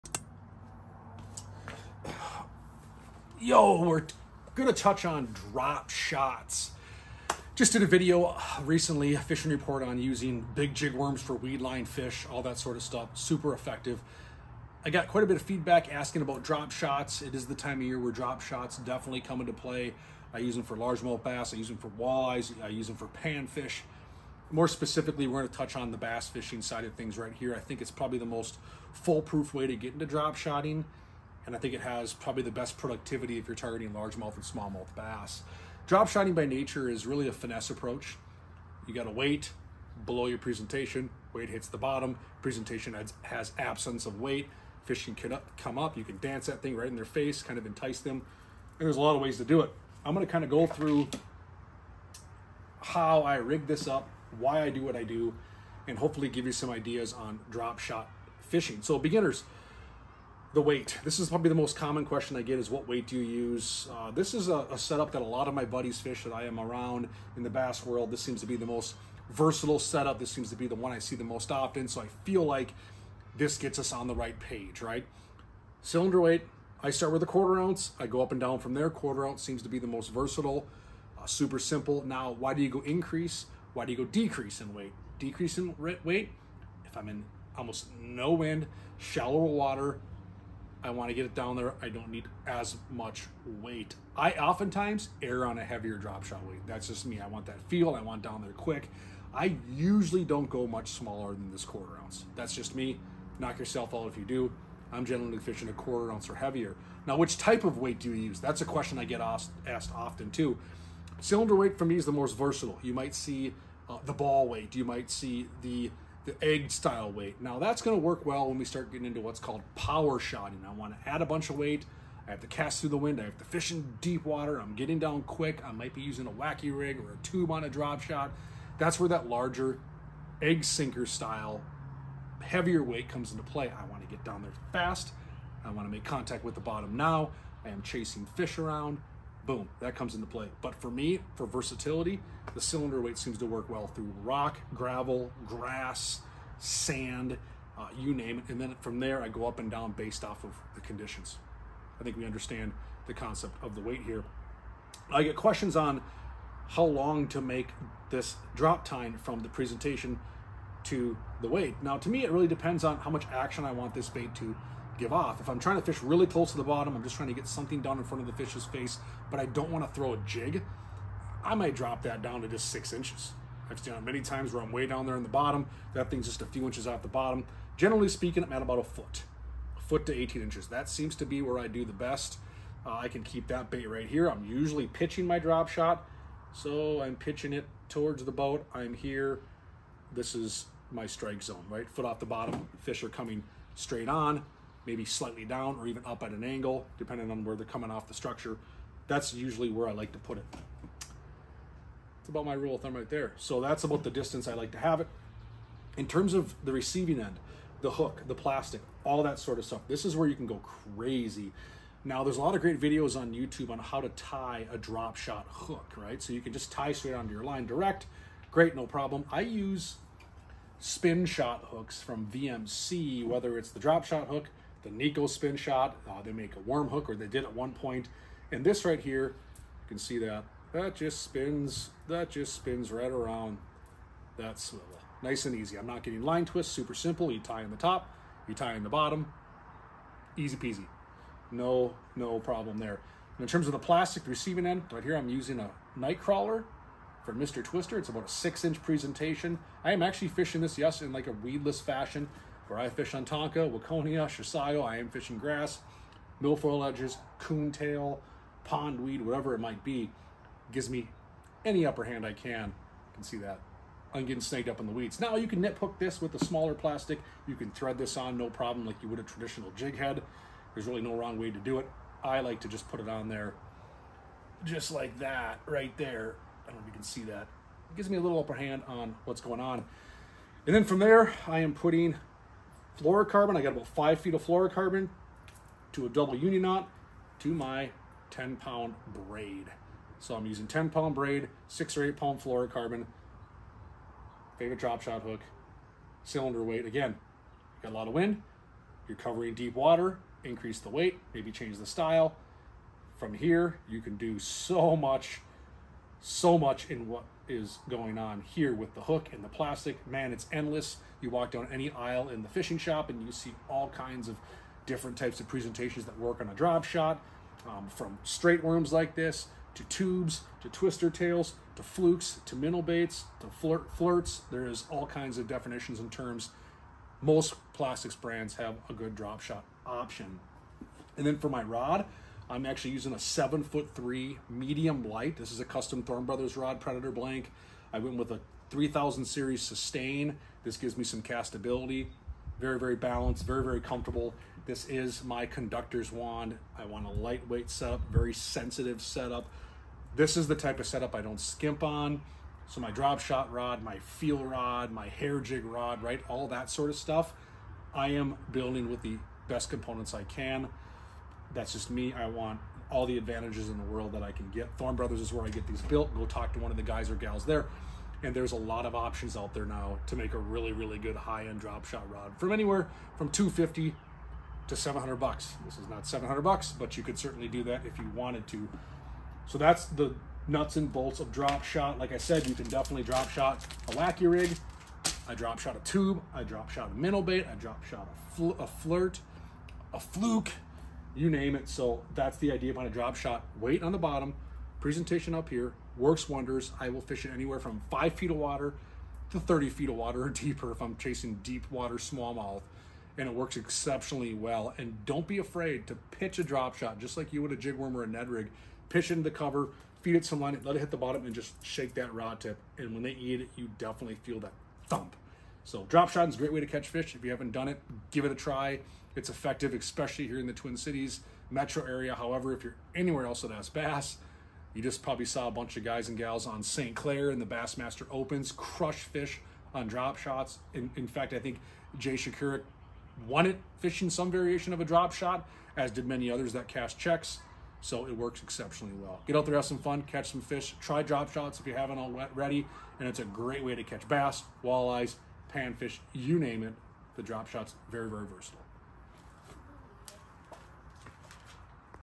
The Drop Shot Rig Breakdown | Bass Fishing Setup
The drop shot rig doesn’t have to be intimidating and can be very productive for bass, walleye, panfish… you name it! Here, we will focus on bass and go over why/how it makes perfect sense on summertime bass!!

0:00 Intro
0:35 What is a Drop Shot
1:00 Which Drop Shot Weight to Use
2:35 How Long of a Drop to Use
3:26 The Complete Setup - Hook, Line, Rod and Reel
6:20 Summary of the Drop Shot Rig